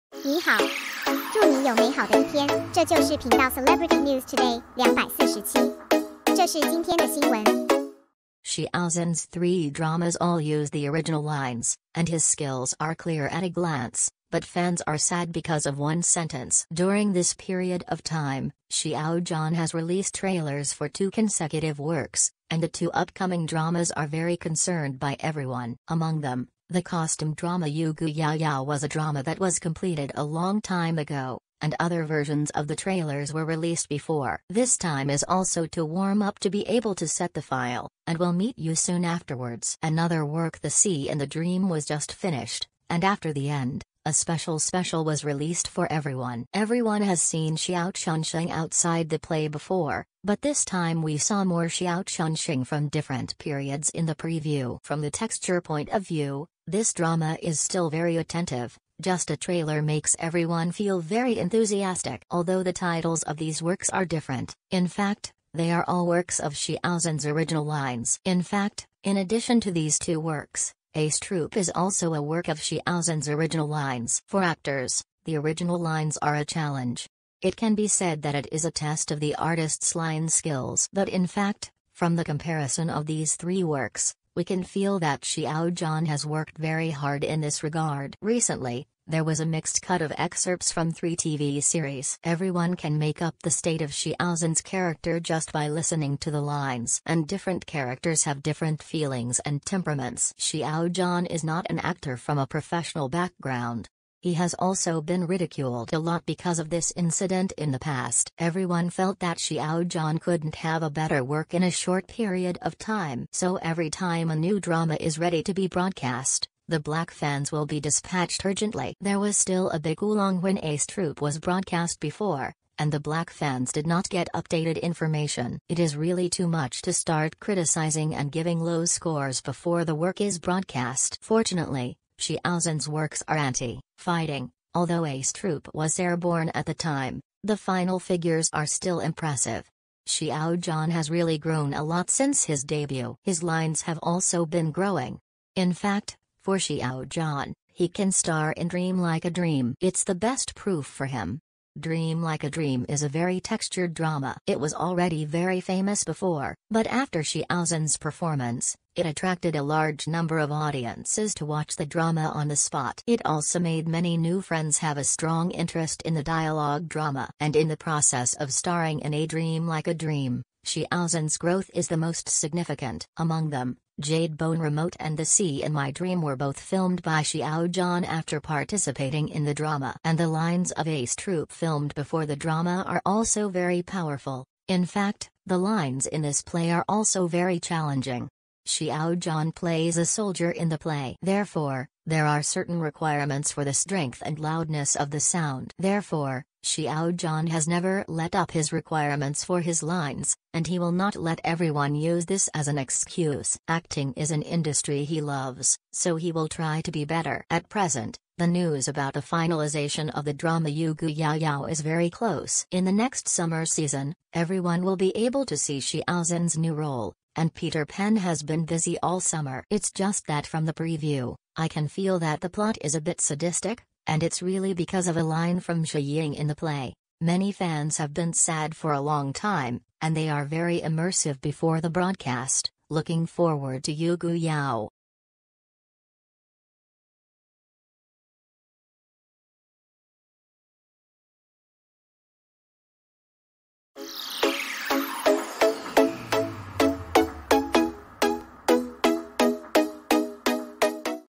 Xiao Zhan's three dramas all use the original lines, and his skills are clear at a glance, but fans
*Celebrity News Today 247 2022/06/23 : Xiao Zhan's three dramas all use the original lines, and his skills are clear at a glance, but fans are sad because of one sentence. ...